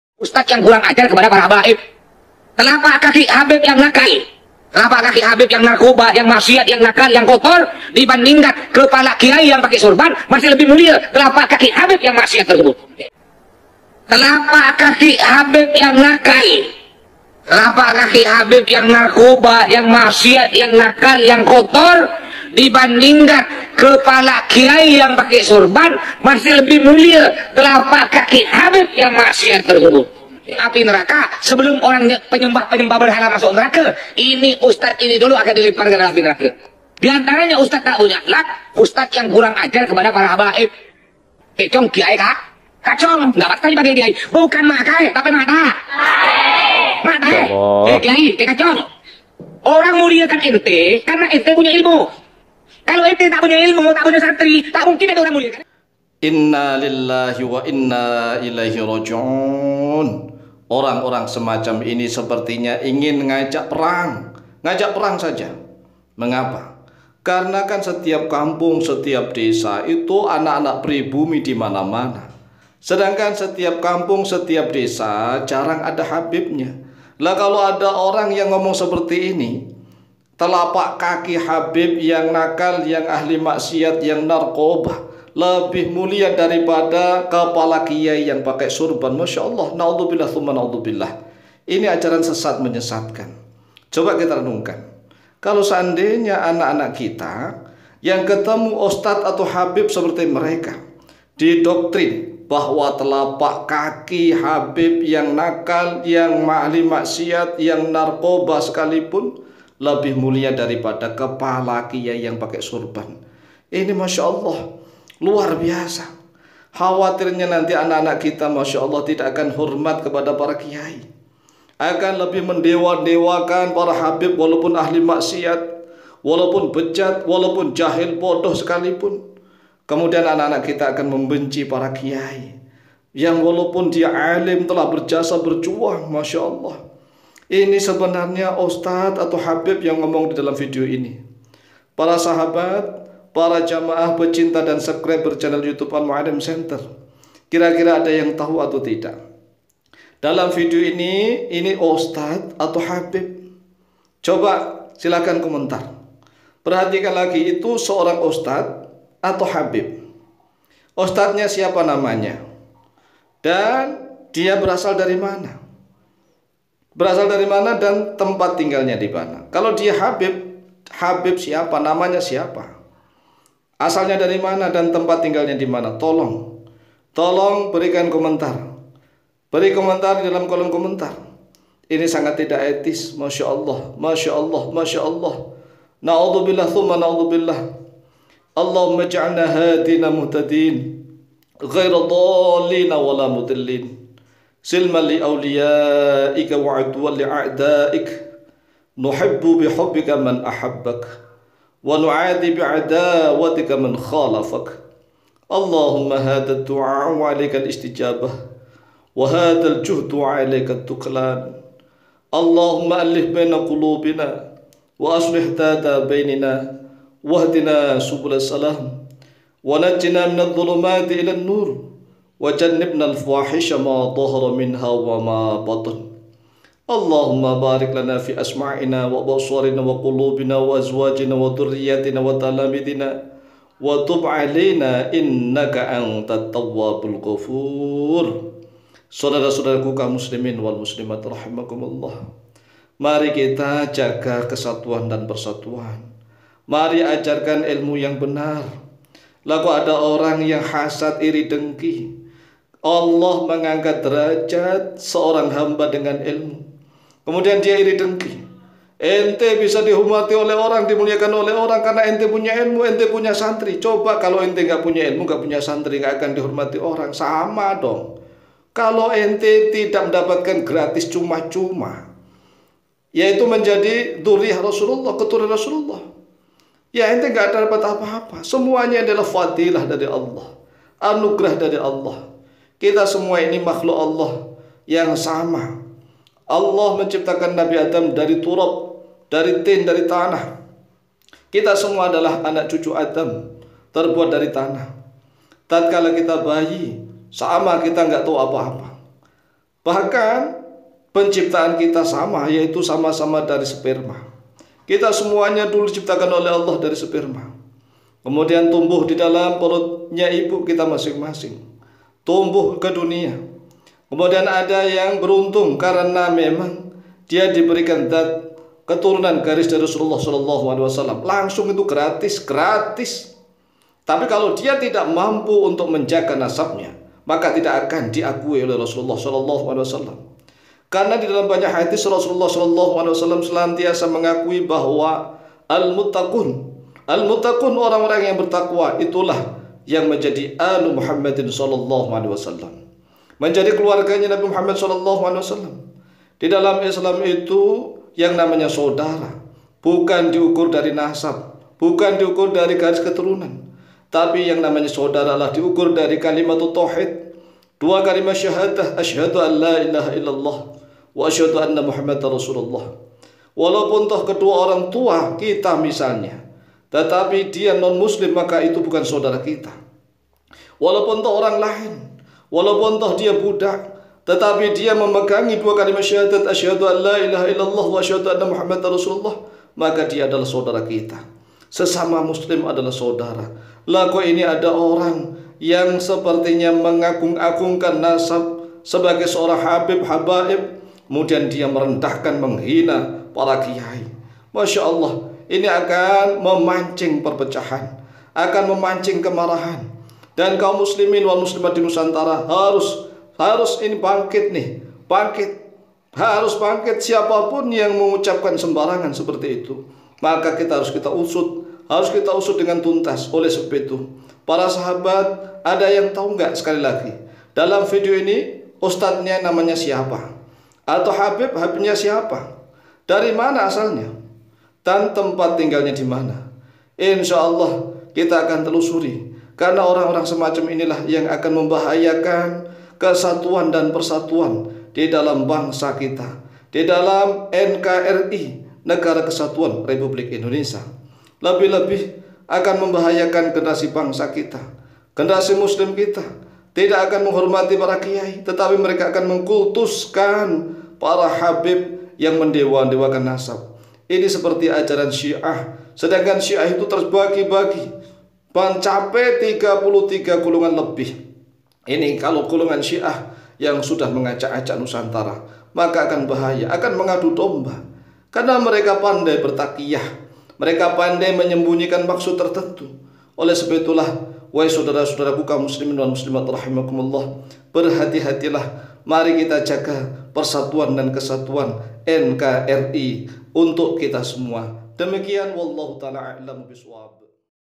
Telapak Kaki Habib Maksiat Telah Berani Injak-Injak Kepala Para Kyai Bersorban
Bagi para sahabat yang tahu nama orang dalam video ini, maka tolong infokan kepada kami. Terimakasih